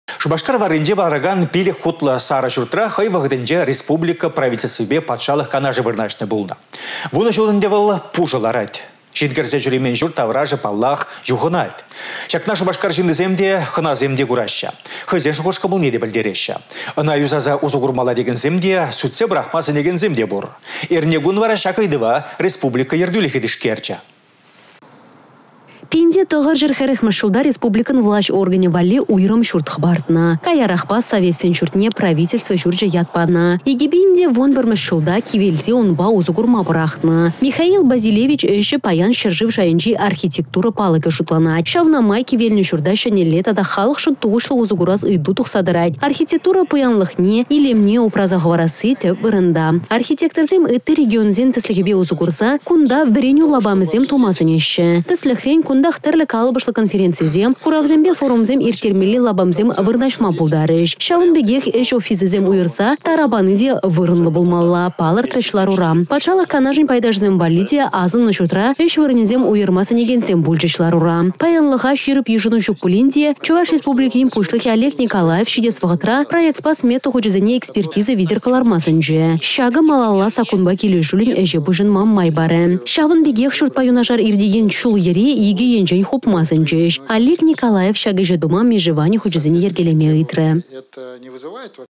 Кивĕ çурт çĕнĕлле усă кÿрĕ
Шупашкар варринче ларакан пилĕк хутлă сарă çуртра хăй вăхăтĕнче республика Правительствипе Патшалăх Канашĕ вырнаçнă пулнă. Вунă çул ĕнтĕ вăл пушă ларать. Çын кĕрсе çӳремен çурт-таврашĕ паллах юхăнать. Çакна Шупашкар çыннисем те, хăнасем те кураççĕ. Хăйсен шухăш-кăмăлне пĕлтереççĕ. Ăна юсаса усă курмалла текенсем те, сӳтсе пăрахма сĕнекенсем те пур.  Эрнекун вара çак ыйтăва республика ертӳлĕхĕ тишкерчĕ.